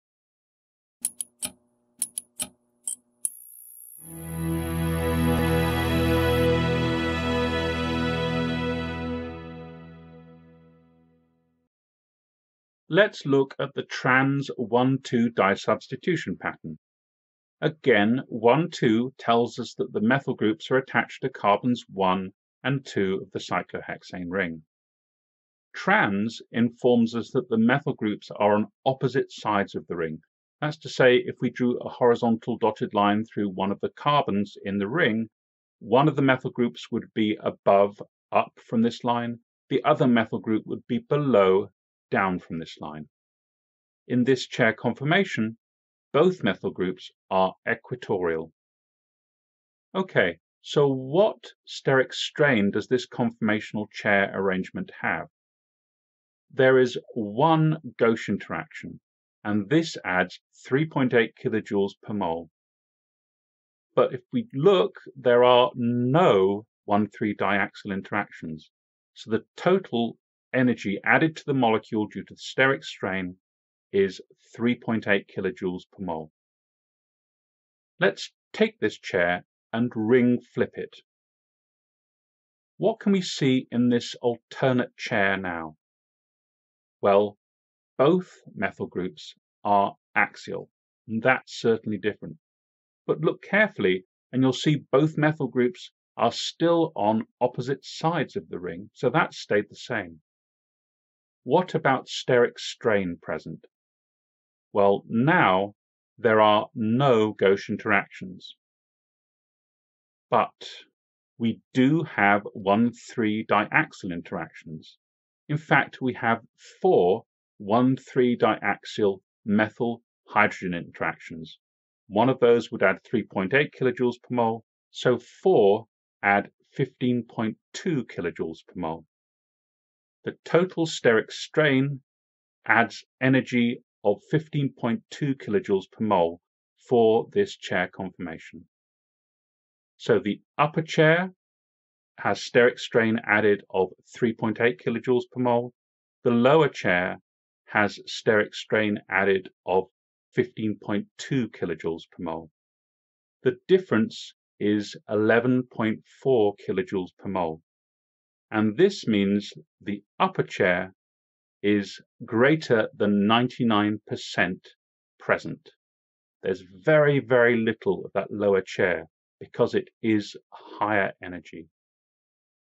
Conformation: Trans-1,2 Disubstitution, Energy of the two Chair Cyclohexanes
Summary:
Trans 1,2-Disubstitution has either one gauche interaction and no 1,3 diaxial interactions, where both substituents are equitorial. Alternatively, with the other ring conformation, where both substituents are axial, there is overall higher energy due to there being no gauche interactions but four 1,3-diaxial interactions. Thus the diequitoral chair conformation is more abundant.